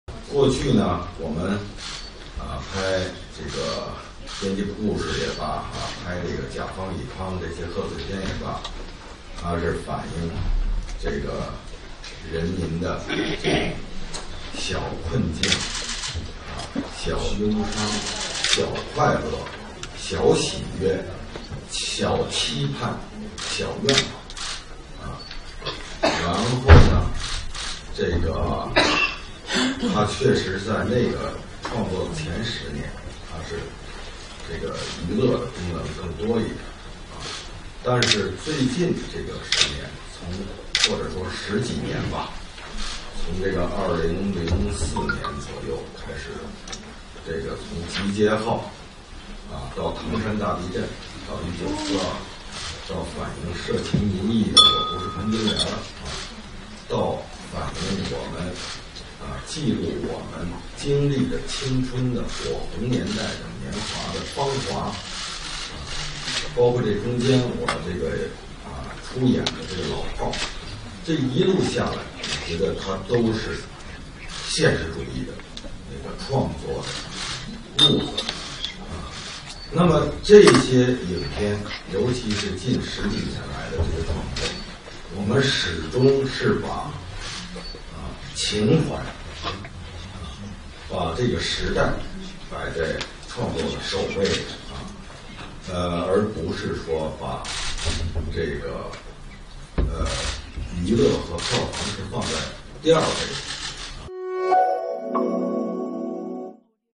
全国政协委员冯小刚：情怀、时代摆在首位 娱乐和票房排第二
3月6日，全国政协委员冯小刚出席全国政协十三届二次会议小组会议。在小组会议中，冯小刚表示，过去是在拍小喜悦、小快乐，近十年拍的《芳华》、《1942》、《老炮》等电影始终把情怀、时代摆在创作的首位，把娱乐和票房放在了第二位。（曹梦雅）